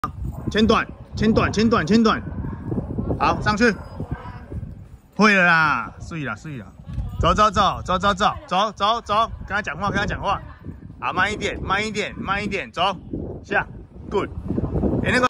有心一定成😄我們是台灣敏捷犬：米克斯/乖乖#訓練有付出一定會有成果🥹
我們是台灣敏捷犬：米克斯/乖乖